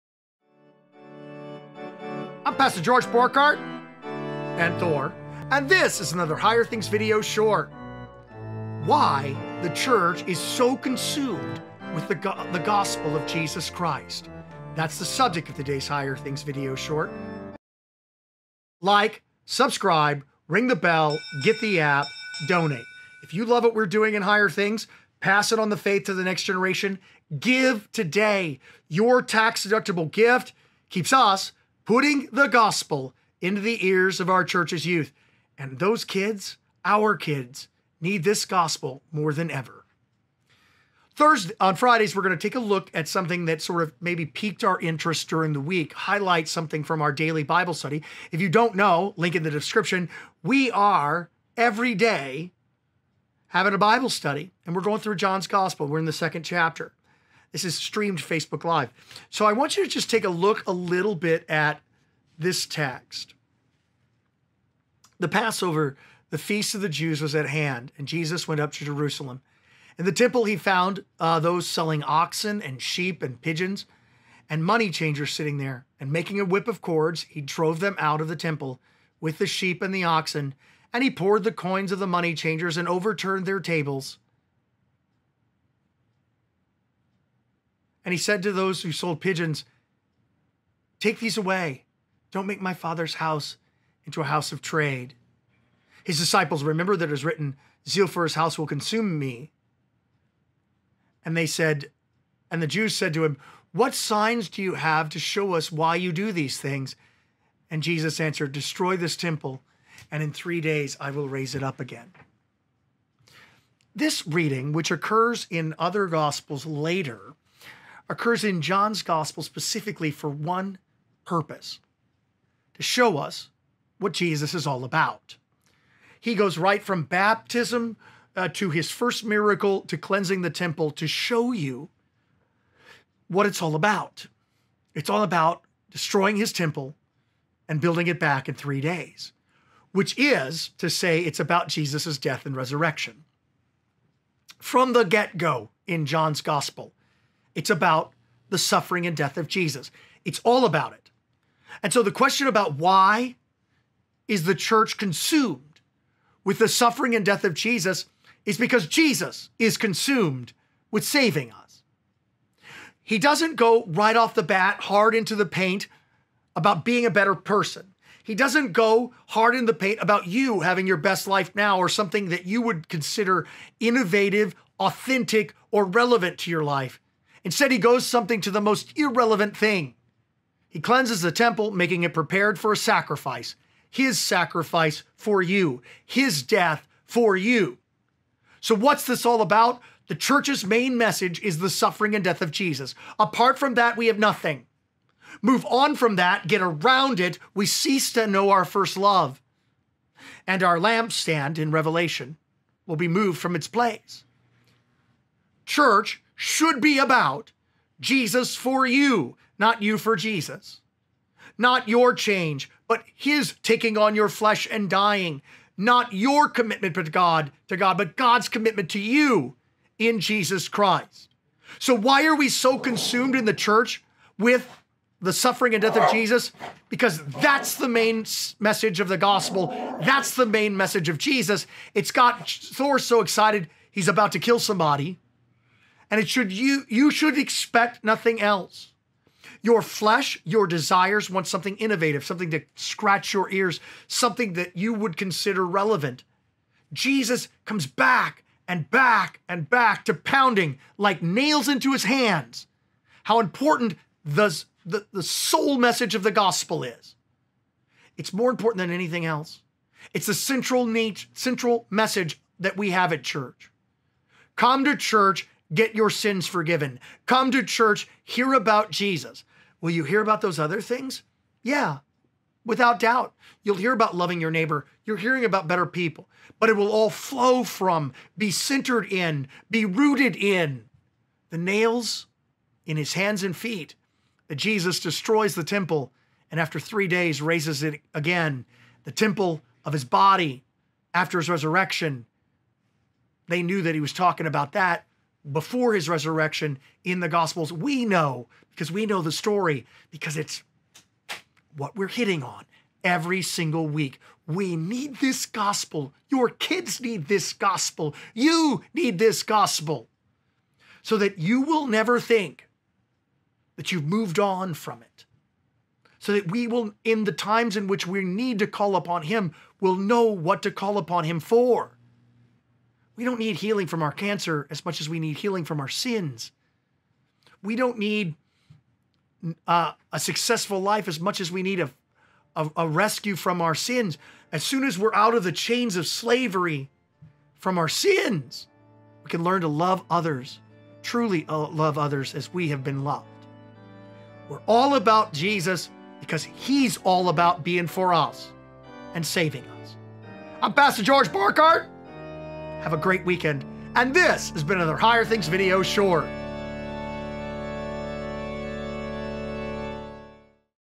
0918 Why The Church Pushes the Gospel So Much - A Higher Things® Video Short
The reason the Church is so consumed and driven by the preaching of the Gospel is simple. It’s because Christ is consumed with saving you, consumed with freeing you of your slavery to sin.